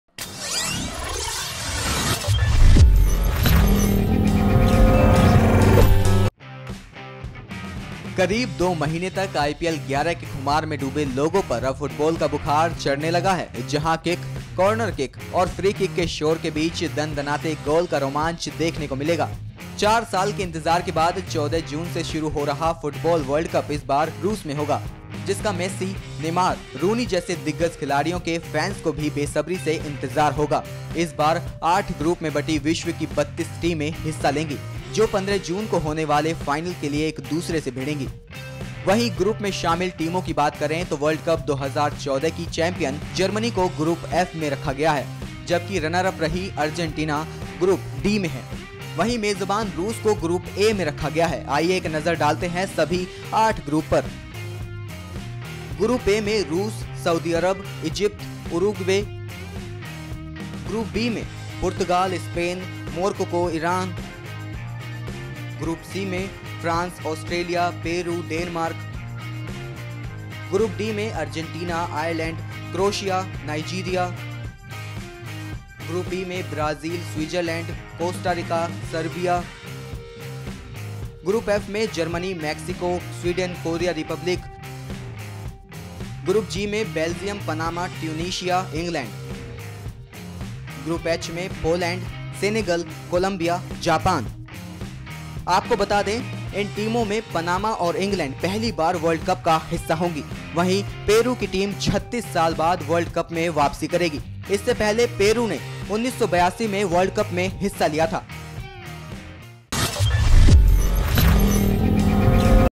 June में शुरू होगा FIFA World Cup, गूंजेगा Goal का शोर
FIFA Football World Cup इस बार 15 June को शुरू हो रहा है, इस Tournament में 8 Groups में बंटी 32 Teams होंगी शामिल

PRODUCER - DHEERENDRA
EDITOR - VINEET